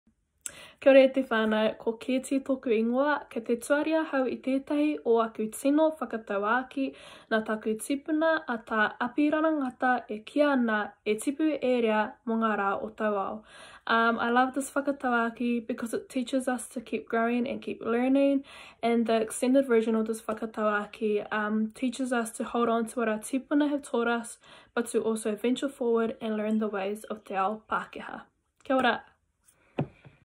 Mahuru Māori 2021 - Keeti's Tino Whakatauākī
E tipu, e rea mo ngā rā o tō ao✨⁣
As Keeti explained, this whakatauākī encourages us to keep growing and learning.. 🌱⁣
What have you learnt about yourself, about others or about the world during this lockdown?